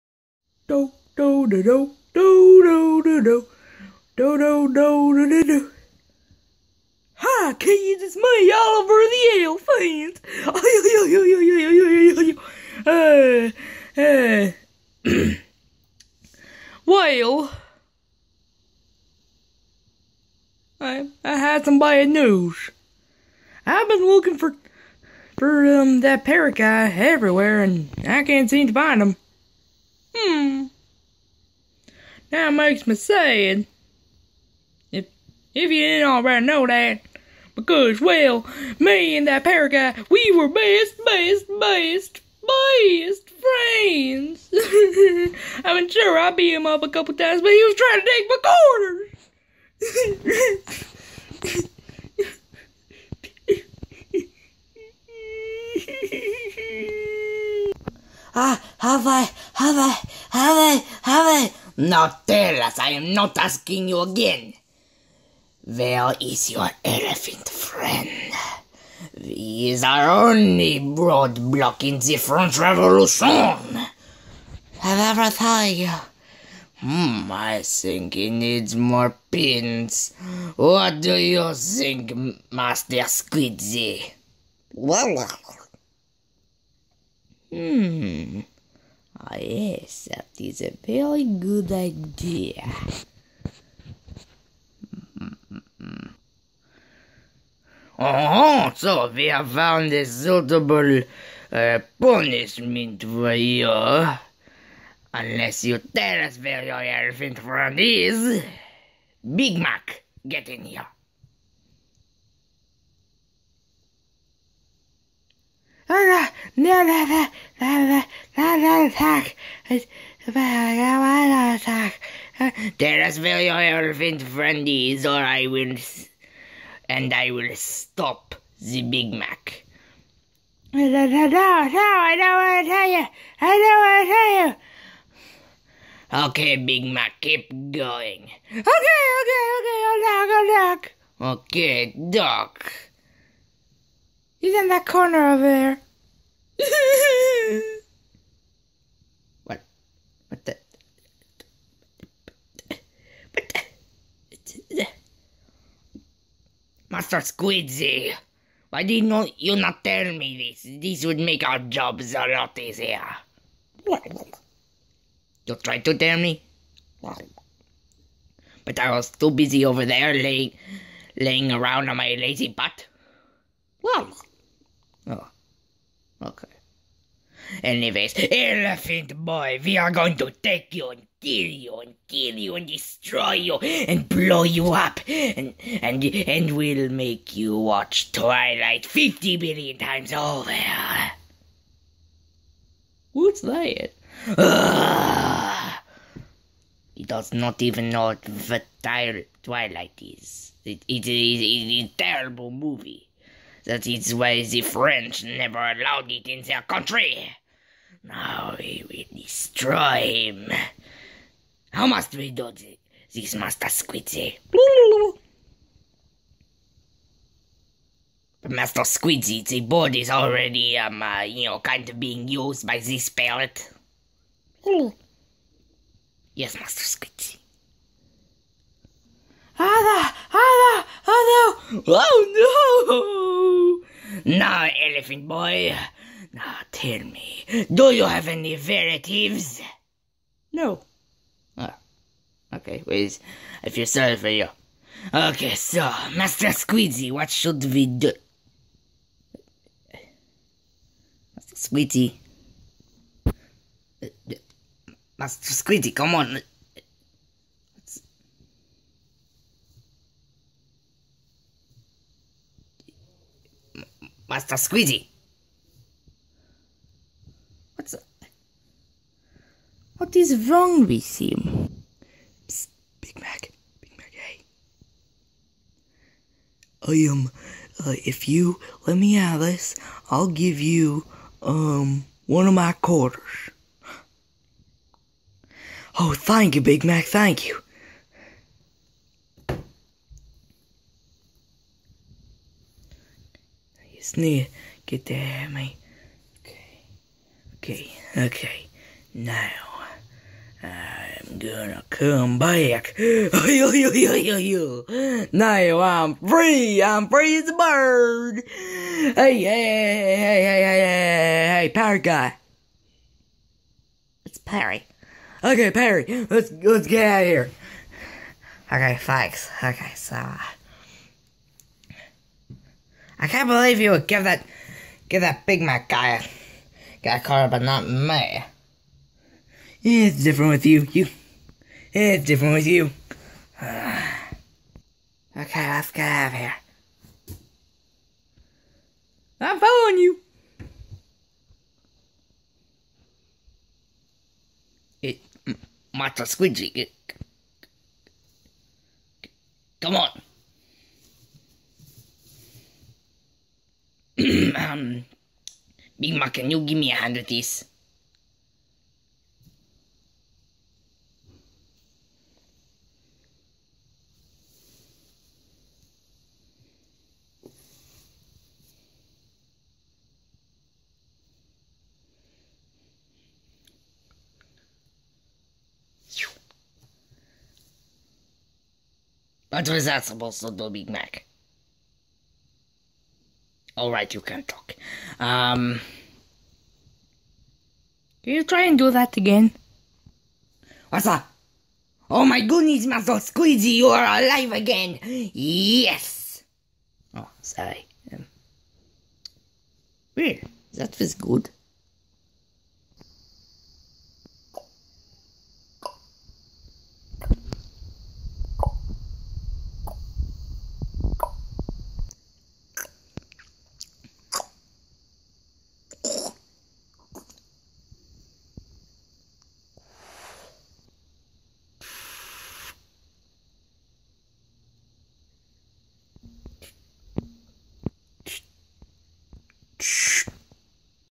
Puppet show 2: parry gets kidnapped!
Since everyone loved the last puppet show I made another one! Who knows I might make a series out of this!